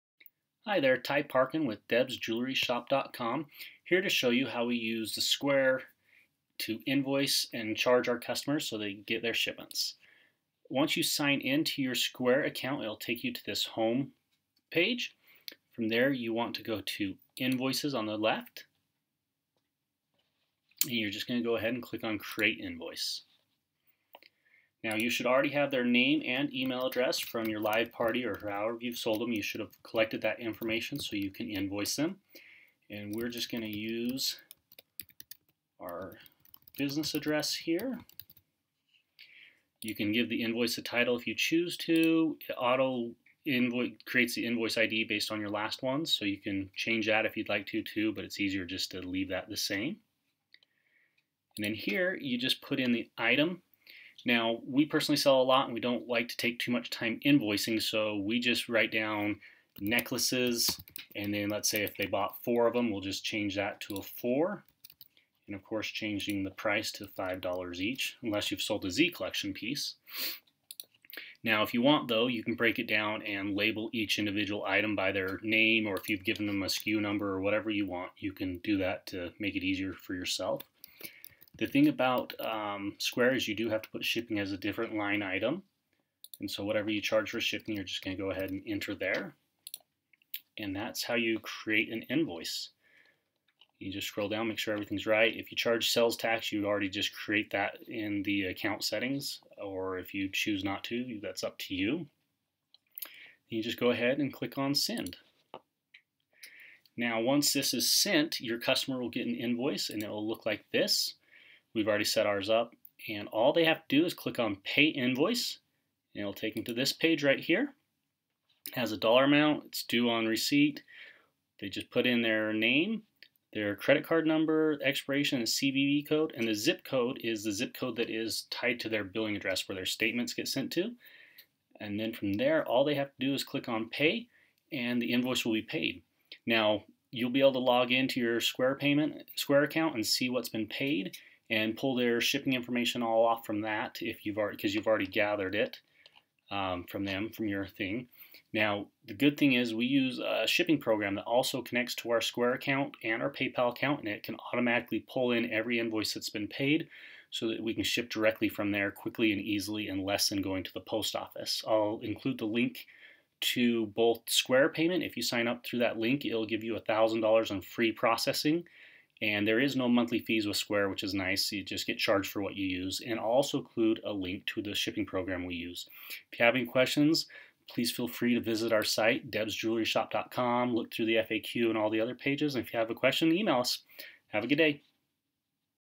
How to invoice our Paparazzi Jewelry Customers with Square
Learn how I invoice my Paparazzi Jewelry Customers with Square.  For more information about Paparazzi Accessories please visit my

I talk about the program I use to ship in this video.  For a free 30 day trial of that program